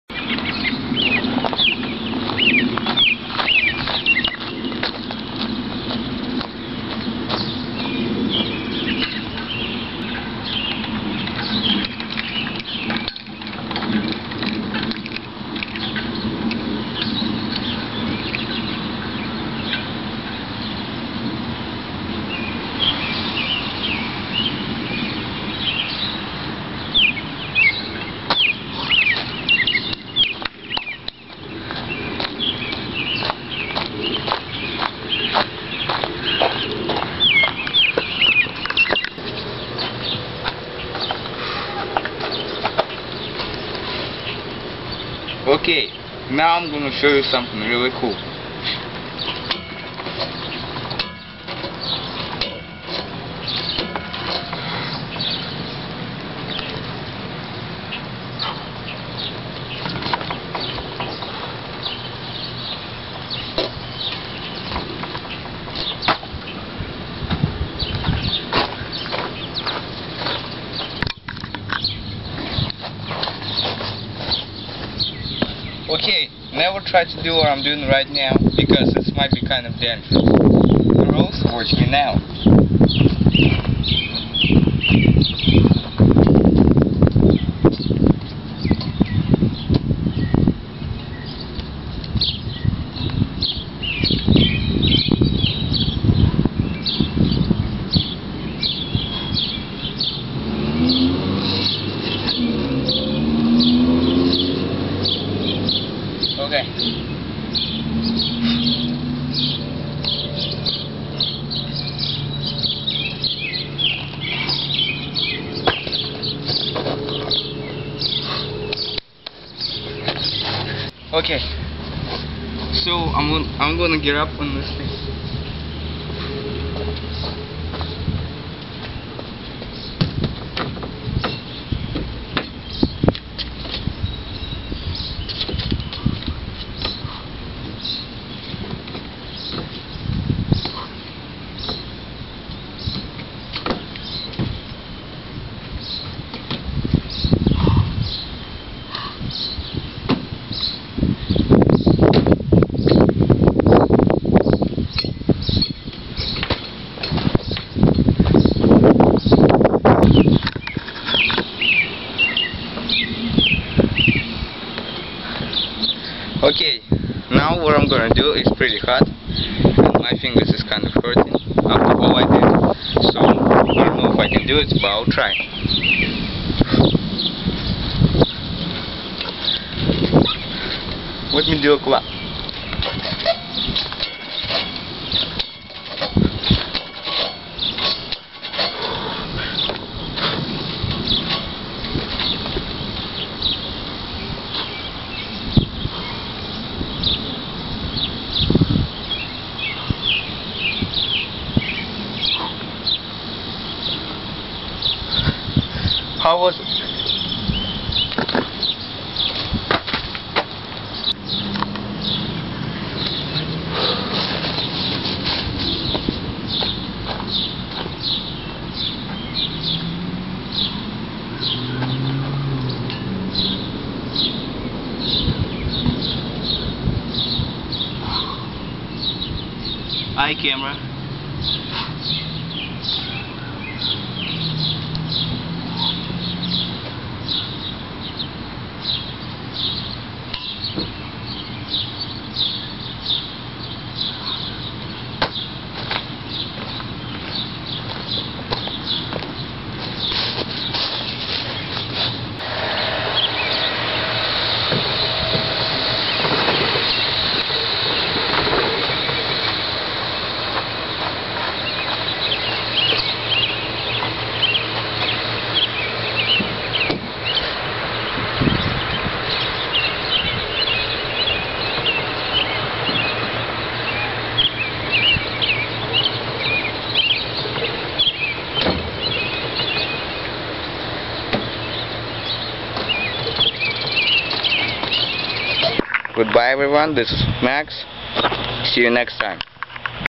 Crazy Max)))
Summer is boring. Many times you don't know what to do. So in my case I just go to the playground in my neighborhood and making some extreme stuff. This is my way of pulling out the energy.